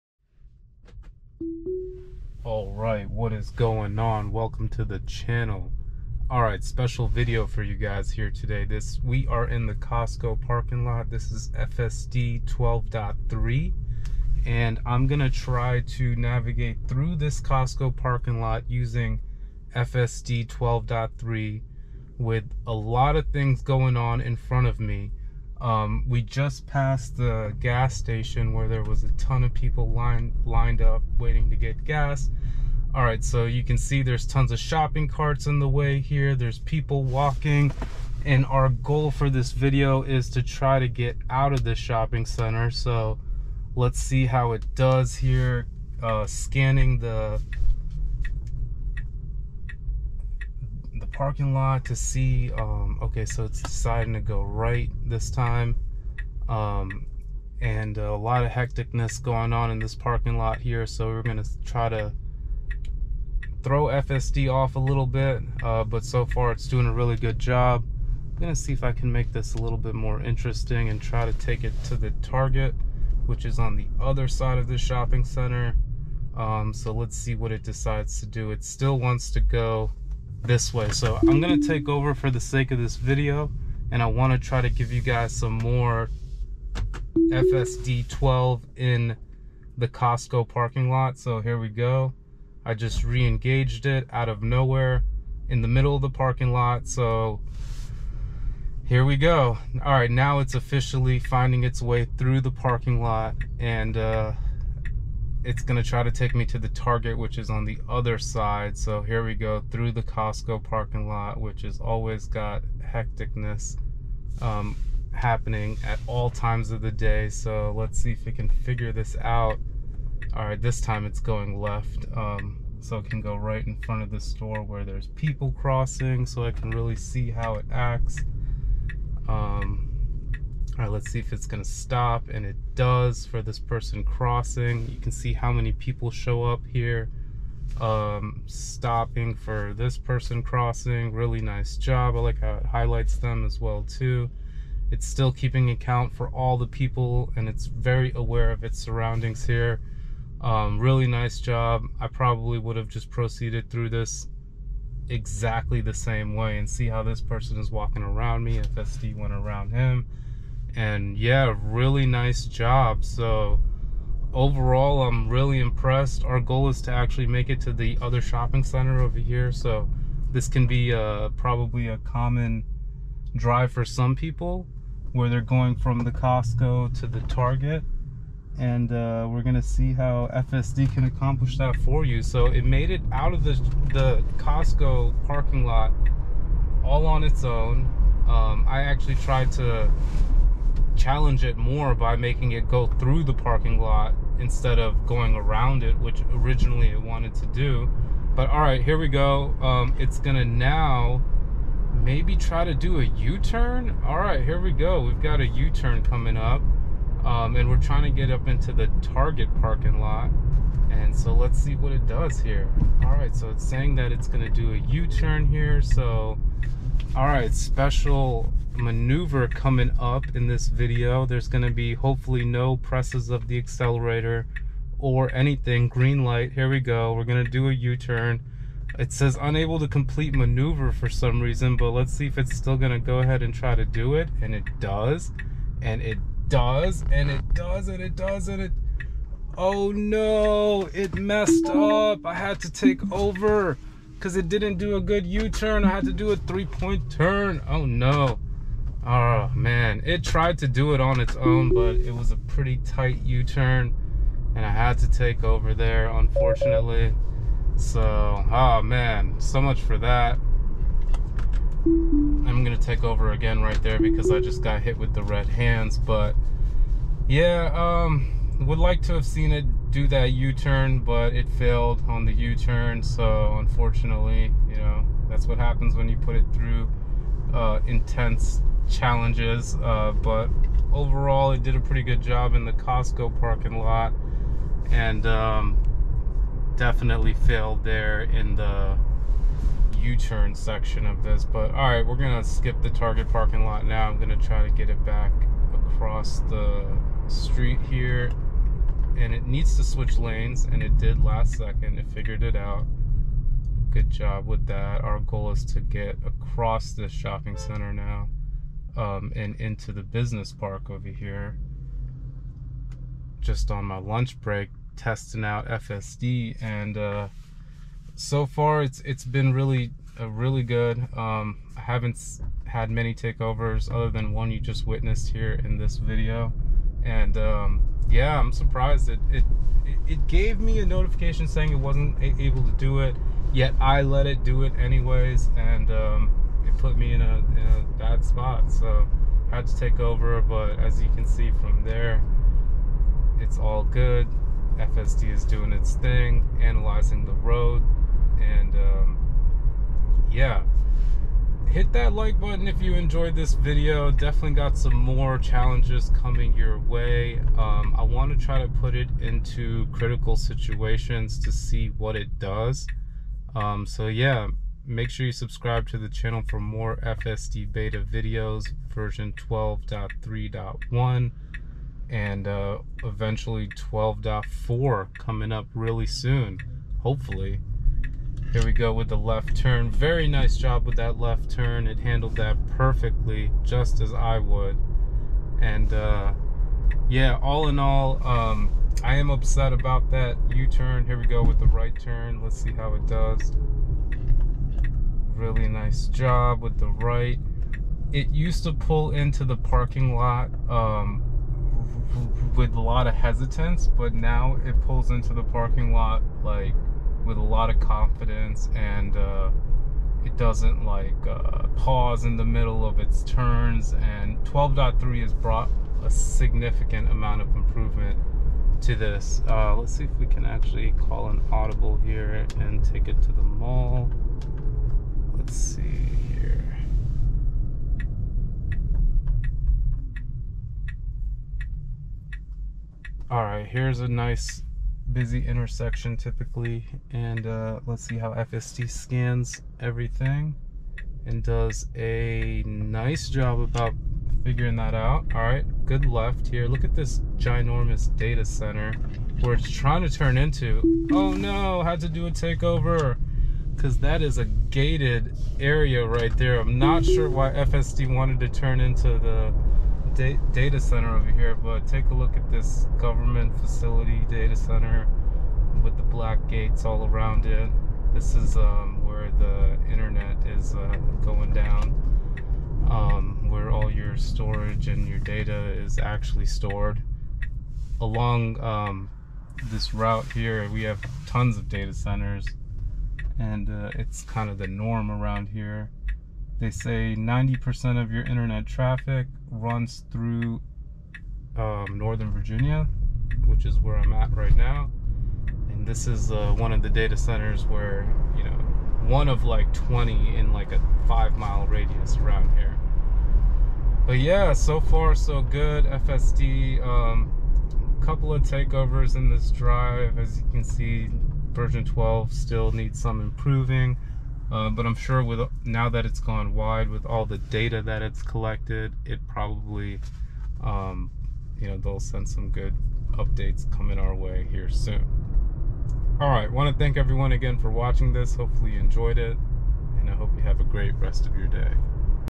Costco Parking lot vs Tesla FSD 12 Real world Artificial Intelligence
Witness the Chauffer skills of FSD v12.3 as I take it on a spin for the first time to see what its all about and how they Implemented AI into its code

2tb SanDisk Sentry upgrade

Tesla Experience
Enhanced Autopilot
Traffic Visualizations
Computer Vision